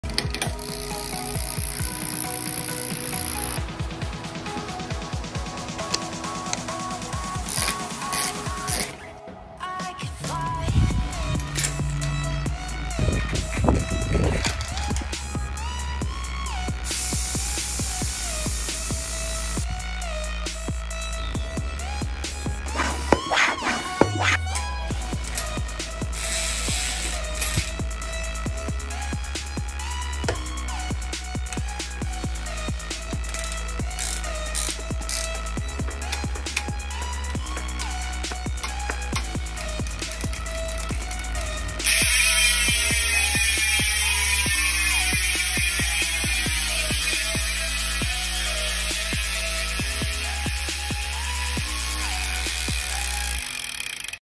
BMX wheel swap. Colony wasp hub. Oil slick jet fuel.
Swapping the rear wheel on an oils slick BMX with a colony wheel with colony wasp oil slick hub.